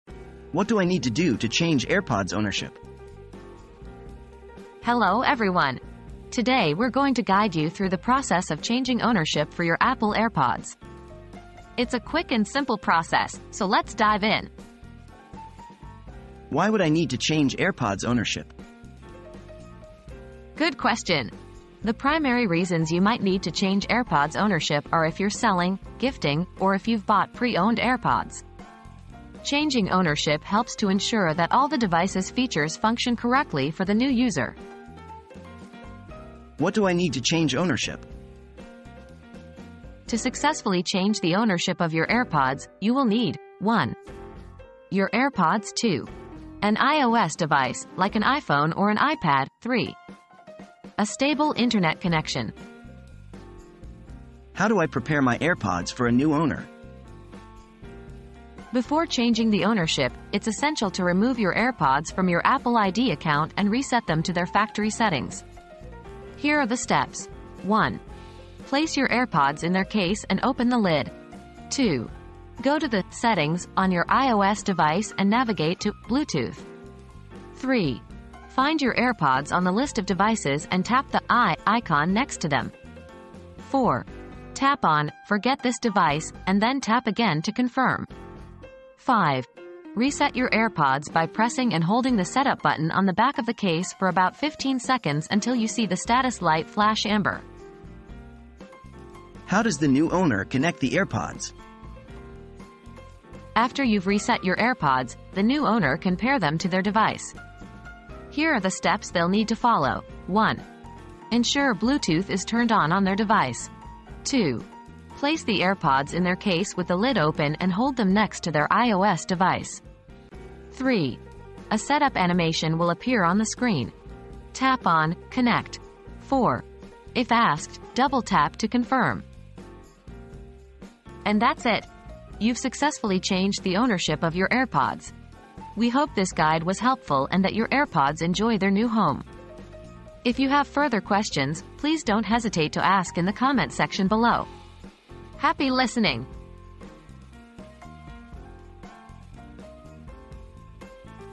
What do I need to do to change AirPods ownership?
How to Change Ownership of Your AirPods • Learn how to change ownership of your AirPods with this quick and easy tutorial! Whether you're selling, gifting, or buying pre-owned AirPods, changing ownership is essential to ensure all features function correctly for the new user. Follow our step-by-step guide and get your AirPods ready for their new owner in no time.

00:00 • What do I need to do to change AirPods ownership?
00:16 • Why Would I Need to Change AirPods Ownership?
00:36 • What Do I Need to Change Ownership?
00:54 • How Do I Prepare My AirPods For a New Owner?
01:39 • How Does the New Owner Connect the AirPods?